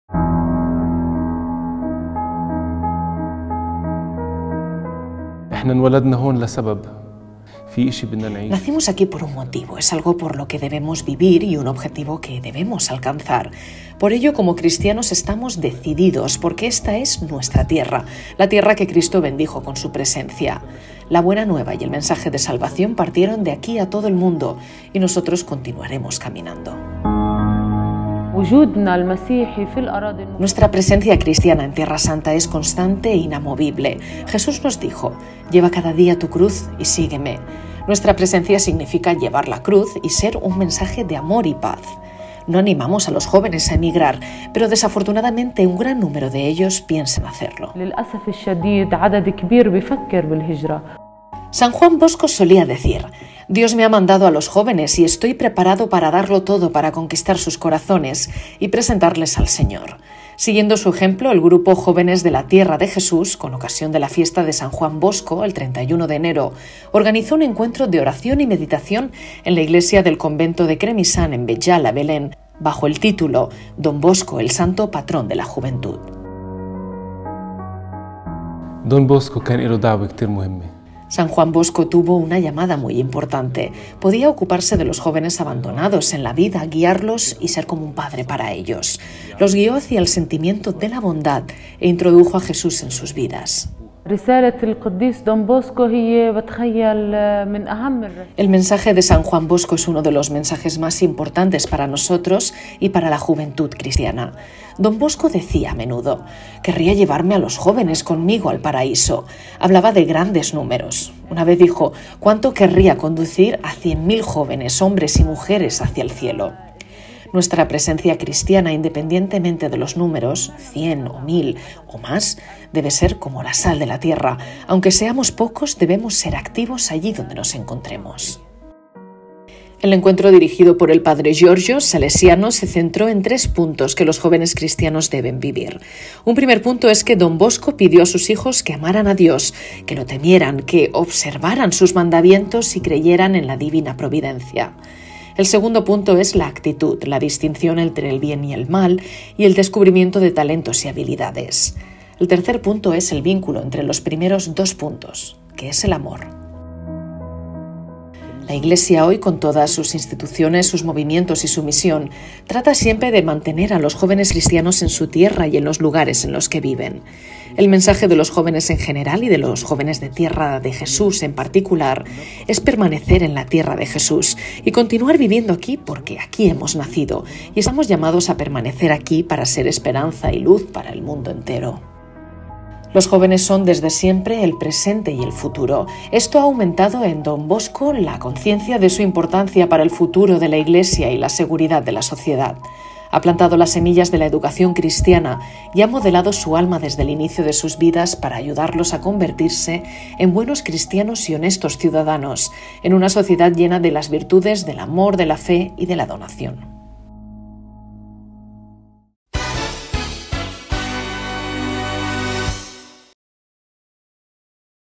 Don Bosco: padre, educador y profesor de la juventud
Con ocasión de la fiesta de San Juan Bosco, el 31 de enero, “jóvenes de la Tierra de Jesús” organizaron un encuentro de oración y meditación en la Iglesia del Convento de Cremisán, en Beit Jala, Belén, bajo el título: “Don Bosco, el santo patrón de la juventud”.